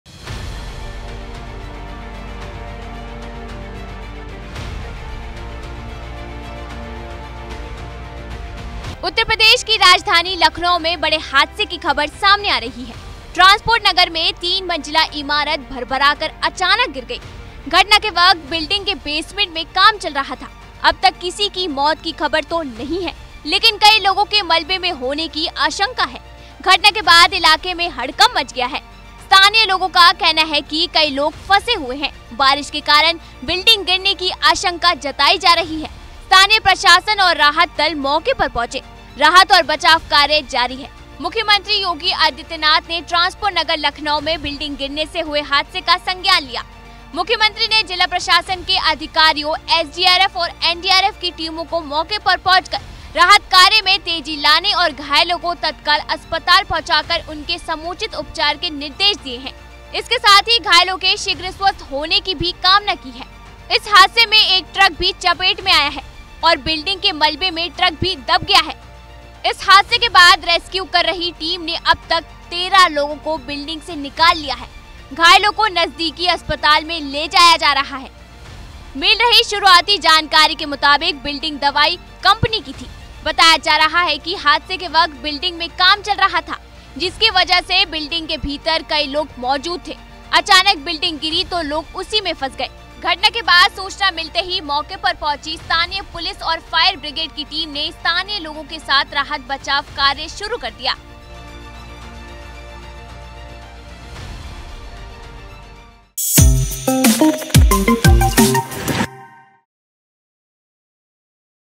Lucknow Building Collapse: लखनऊ के ट्रांसपोर्ट नगर में गिरी इमारत अब तक 4 की मौत |ABP LIVE
राजधानी लखनऊ के ट्रांसपोर्ट नगर में बड़ी इमारत गिरने से बड़ा हादसा हुआ. इस हादसे में कई लोगों के दबे होने की आशंका है. इस घटना में अब तक 4 लोगों की मौत हो गई है और 17 घायल लोगों को अस्पाताल में भर्ती कराया गया. 4 लोंगो की मौत की पुष्टि कमिश्नर ने की है, मरने वालों में दो के नाम पंकज और धीरज हैं. इस घटना के बाद सूचना मिलते ही मौके पर पहुंची स्थानीय पुलिस और फायर ब्रिगेड की टीम ने स्थानीय लोगों के साथ राहत बचाओ कार्य शुरू कर दिया.